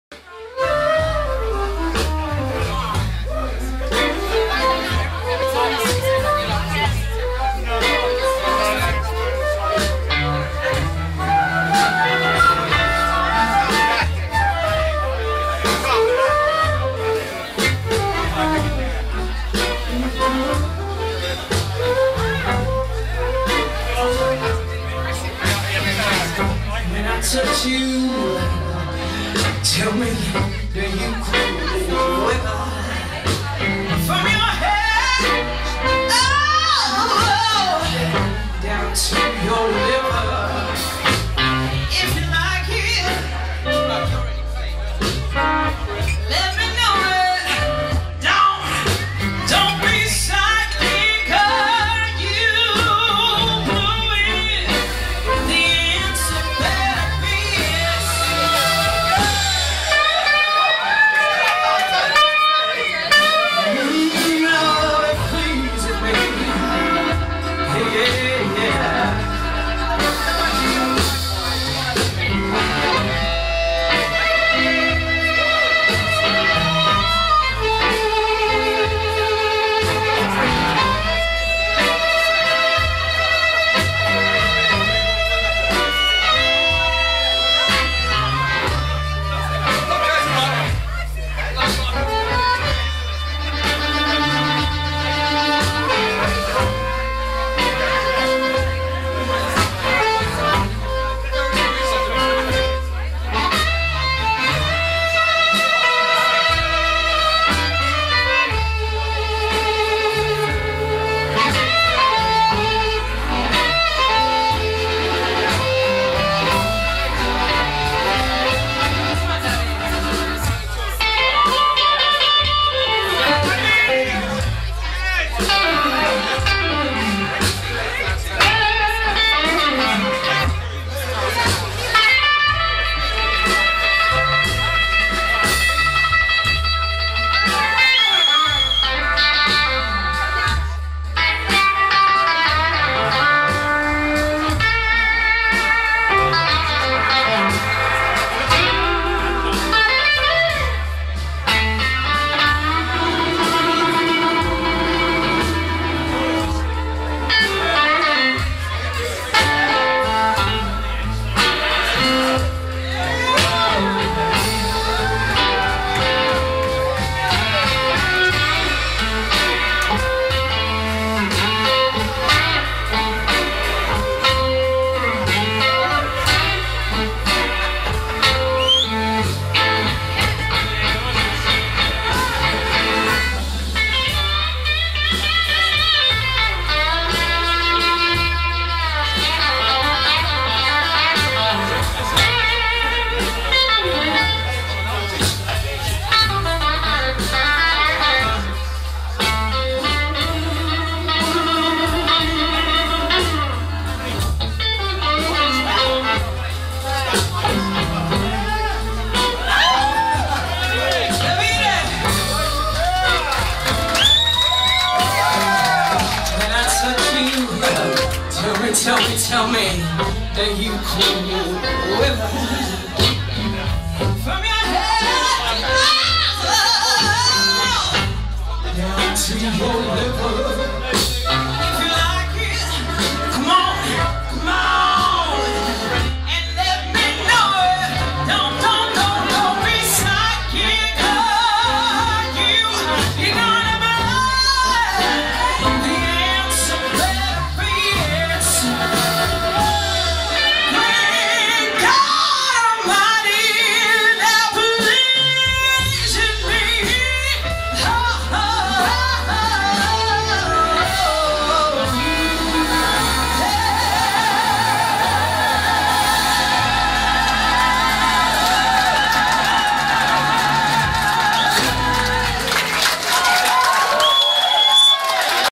London Blues Revue 10 Nov 2017 - Kyla Brox ft Greg Miller
Blues and Soul Diva Kyla Brox at the London Blues Revue. Greg Miller On harmonica, David Mazzantini On guitar. Loris Peverani on drums, Tsukasa Katoaka on bass.